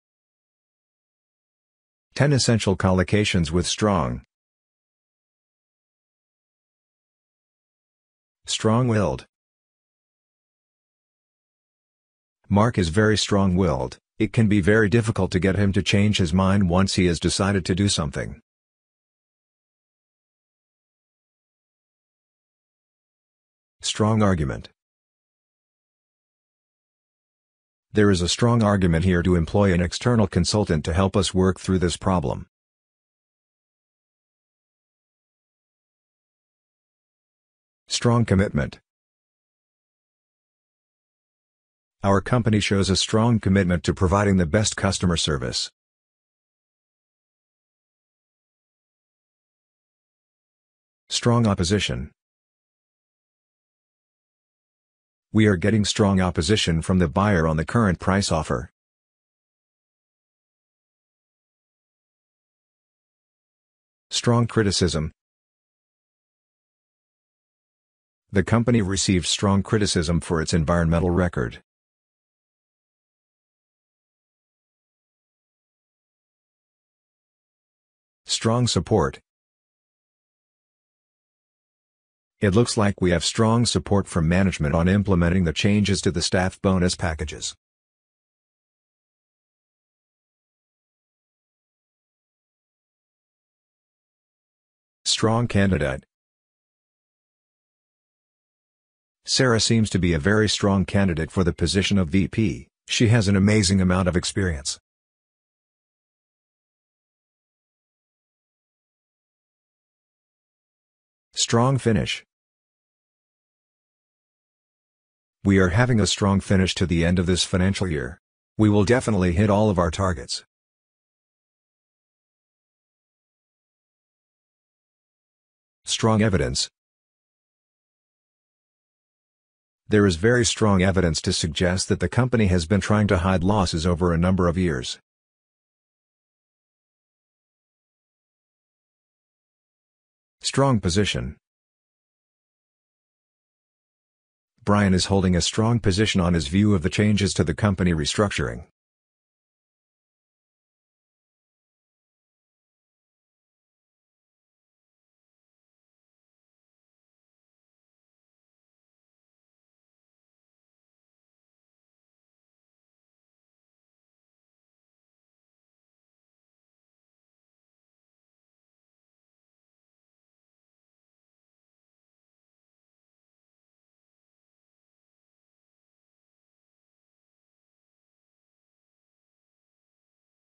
10 Essential English Collocations With Strong | Improving IELTS speaking and writing score
a collocation is a series of words or terms that co-occur more often than would be expected by chance. In phraseology, a collocation is a type of compositional phraseme, meaning that it can be understood from the words that make it up. This contrasts with an idiom, where the meaning of the whole cannot be inferred from its parts, and may be completely unrelated.

An example of a phraseological collocation is the expression strong tea. While the same meaning could be conveyed by the roughly equivalent powerful tea, this adjective does not modify tea frequently enough for English speakers to become accustomed to its co-occurrence and regard it as idiomatic or unmarked. (By way of counterexample, powerful is idiomatically preferred to strong when modifying a computer or a car.)

There are about six main types of collocations: adjective + noun, noun + noun (such as collective nouns), verb + noun, adverb + adjective, verbs + prepositional phrase (phrasal verbs), and verb + adverb.

Collocation extraction is a computational technique that finds collocations in a document or corpus, using various computational linguistics elements resembling data mining.

10 Essential English Collocations With Strong | Improving IELTS speaking and writing